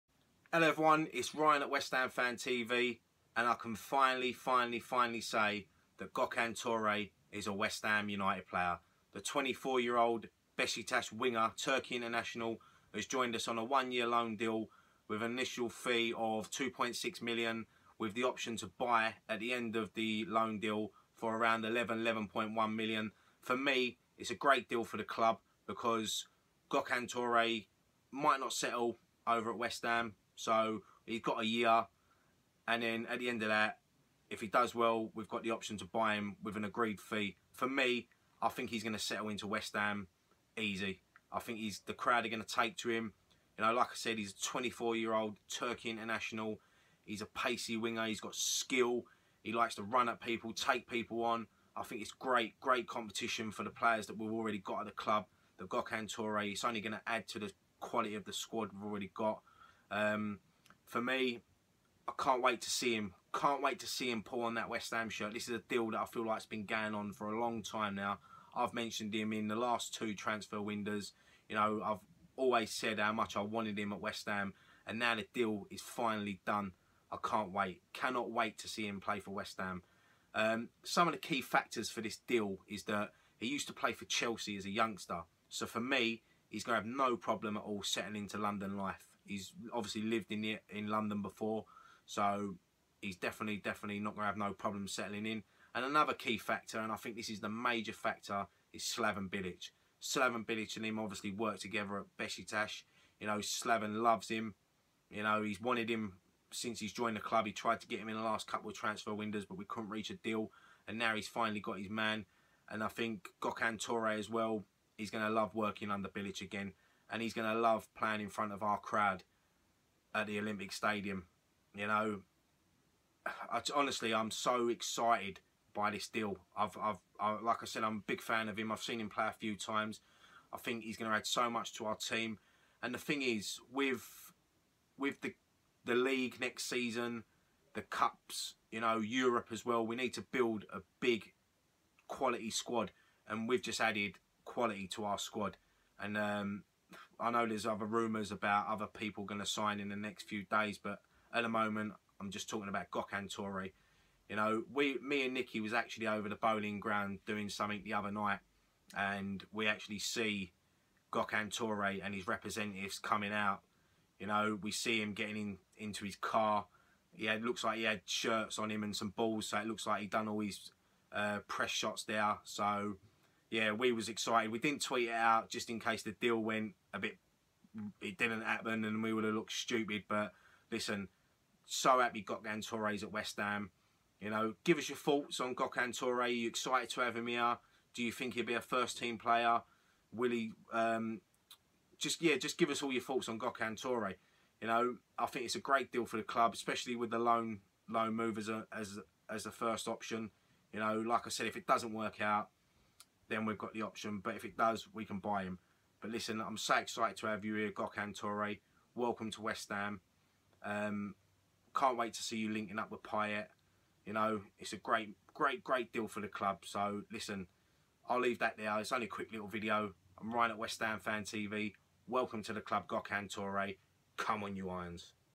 GOKHAN TORE CONFIRMED AT WEST HAM!!!
Ryan discusses the signing of Gokhan tore from besiktas and what strengths the 24 year old Turkey international winger will bring to our squad

Follow un on twitter@westhamfantv
Facebook.com/westhamfantv